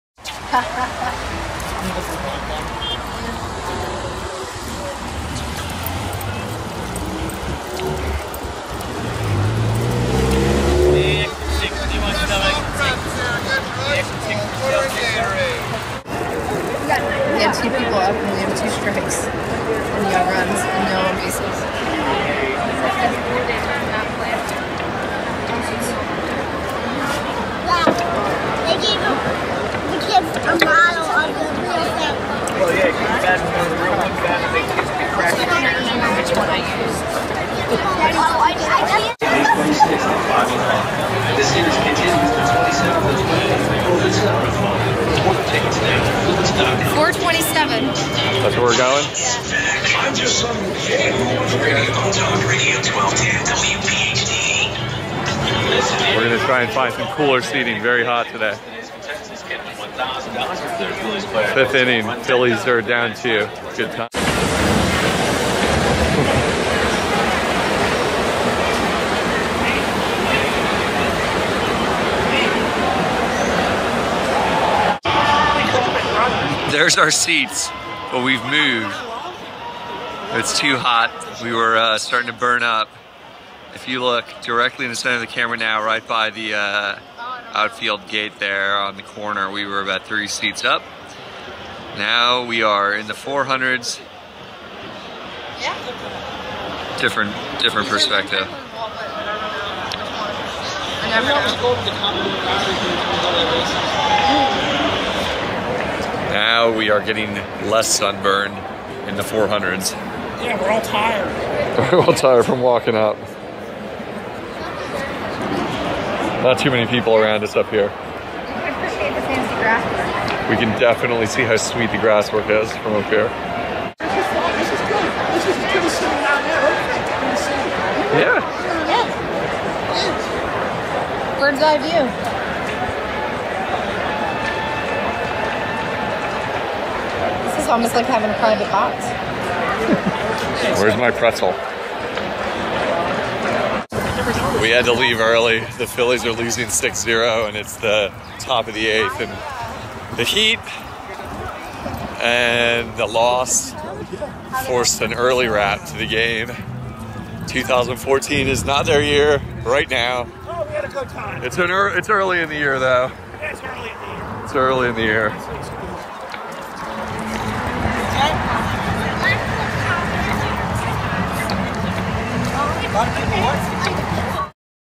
Phillies 25May2014 Rick, Ryan, Tara
Something of a tradition for the past several years, Rick and Ryan (and special guest Tara for 2014) attend a Phillies game for Dad's (belated) birthday gift.  Hot day, Phillies lose, but we still manage to have a good time.